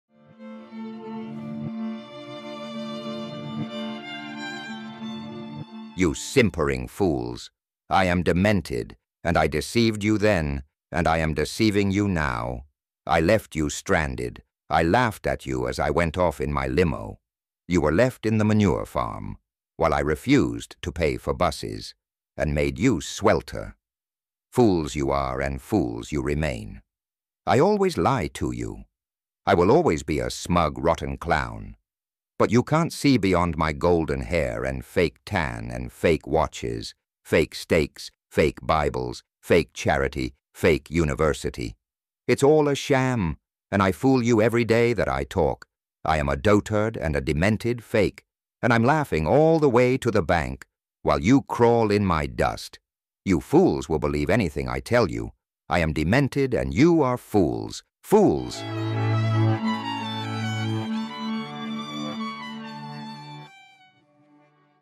DEMENTED AND THE FOOLS
Trump fools some of the people all the time. Demented Donald lies to the fools. AI video